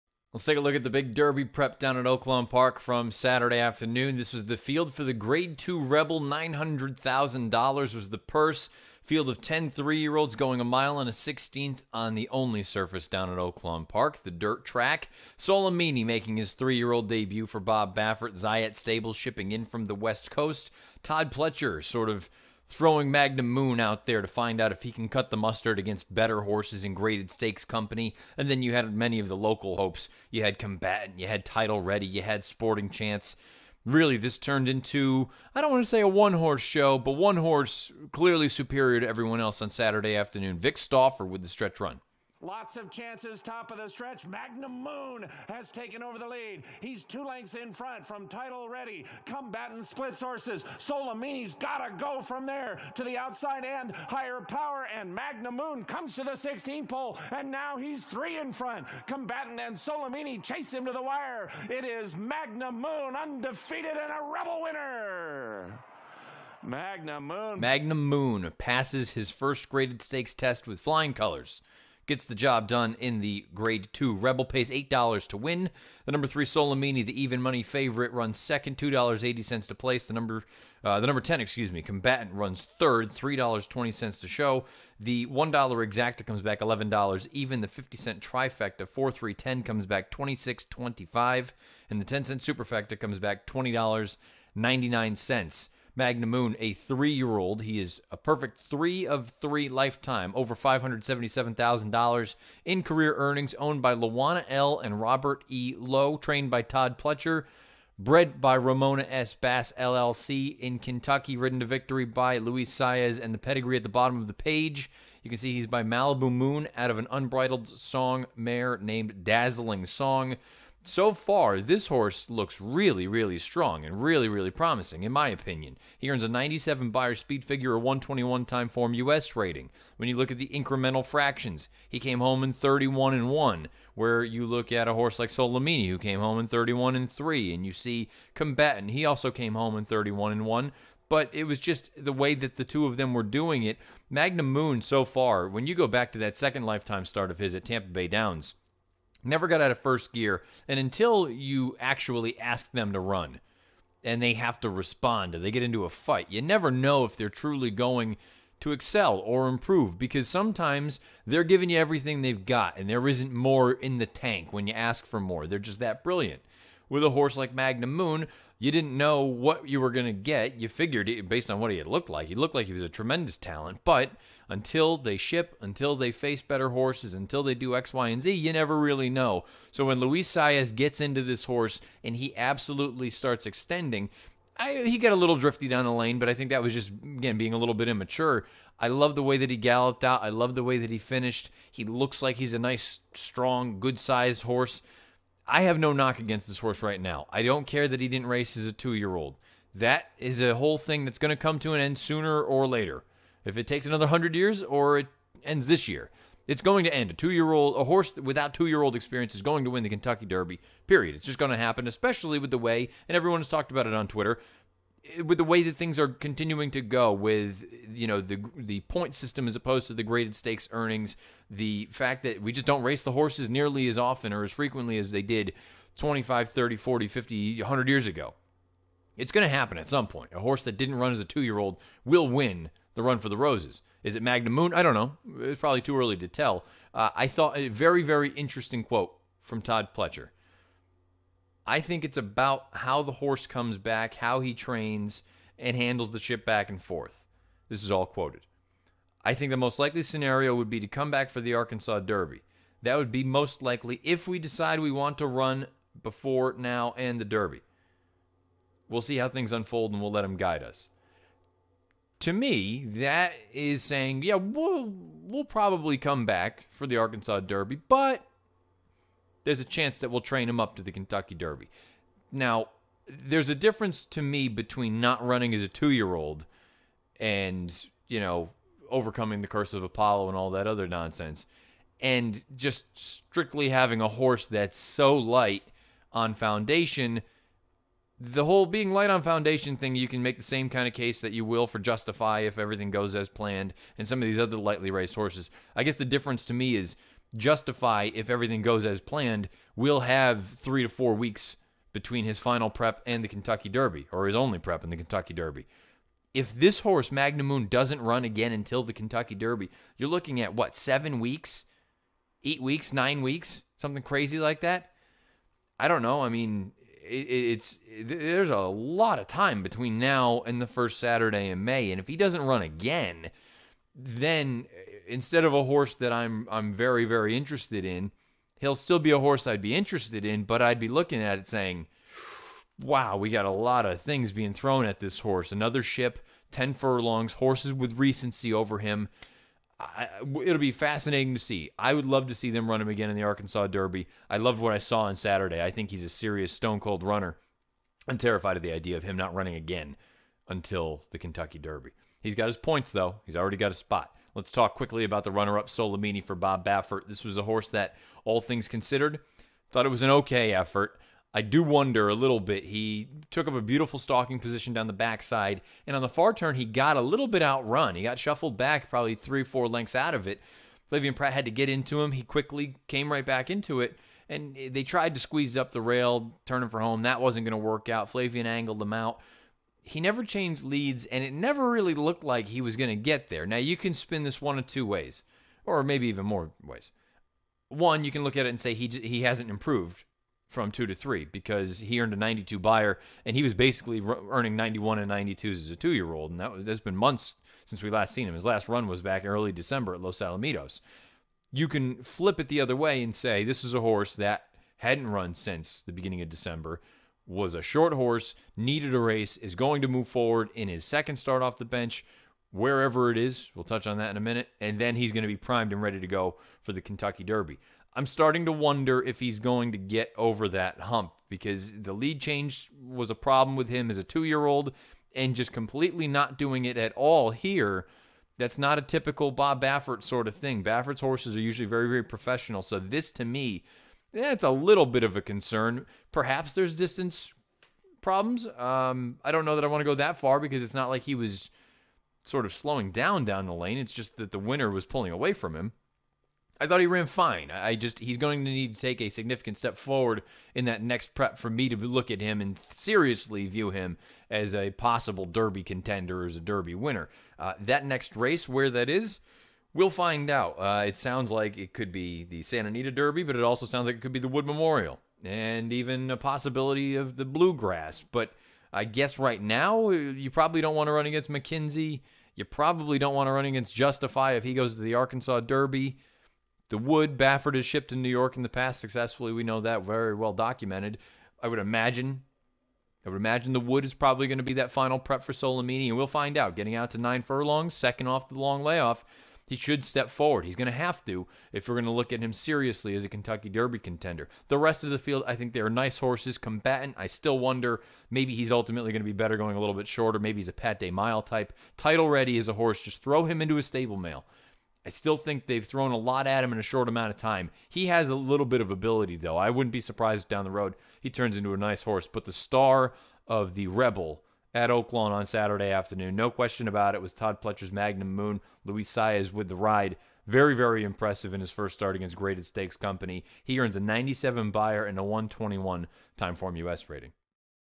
Matt Bernier Recap - Rebel Stakes 2018
Here's Matt Bernier with the recap of Oaklawn Park's Rebel Stakes on DRFTV.